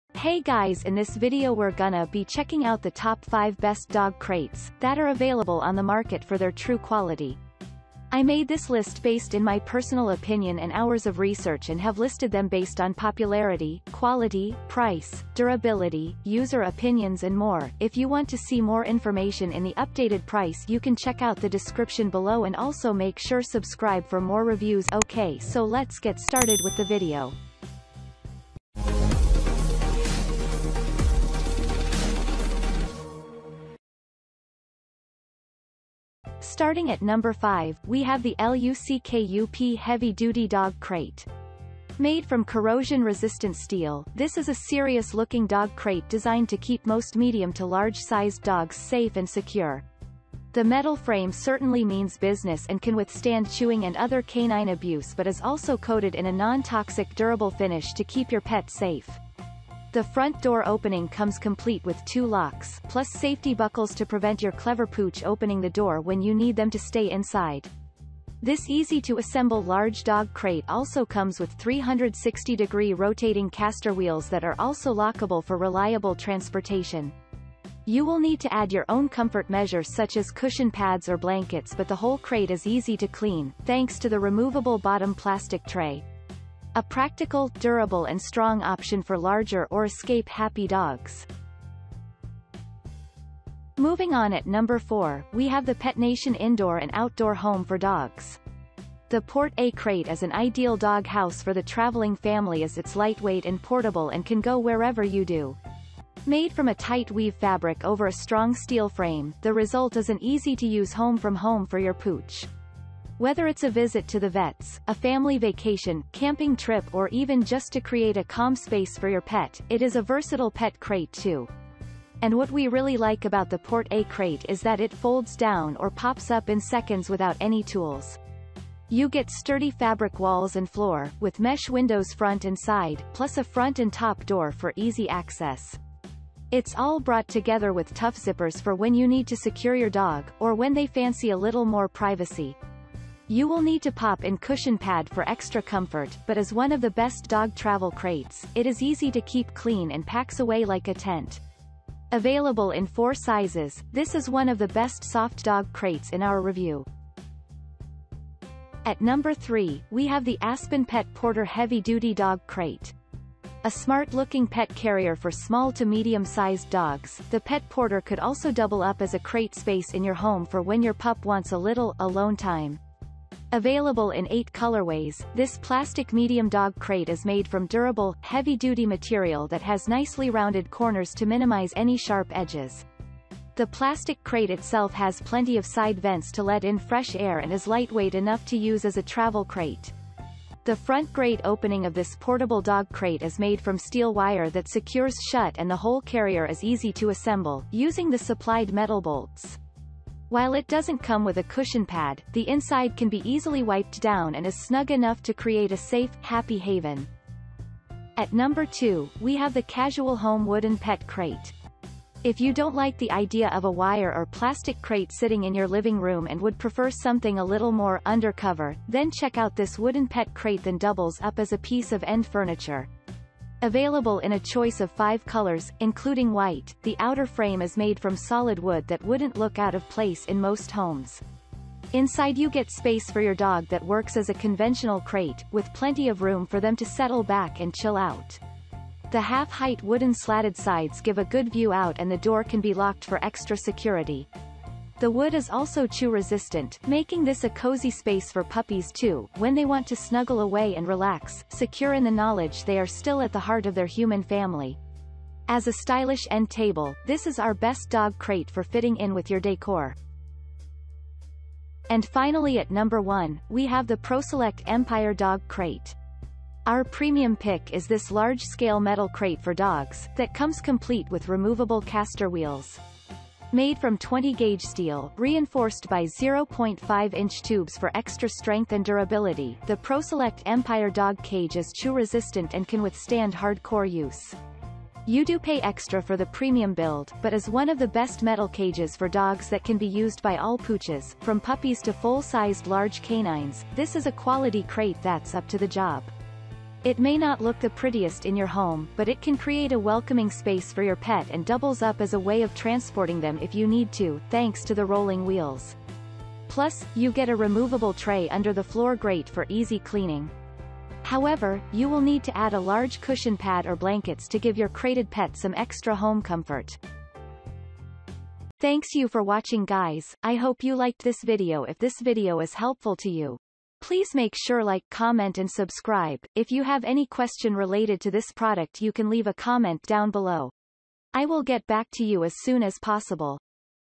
Top 5 Best Dog Crates Review in 2024
▶️ 5. LUCKUP Heavy Duty Dog Crate

▶️ 4. Petnation Indoor and Outdoor Home for Dogs

▶️ 3.  Aspen Pet Porter Heavy-Duty Dog Crate

▶️ 2.  Casual Home Wooden Pet Crate

▶️ 1.  ProSelect Empire Dog Crate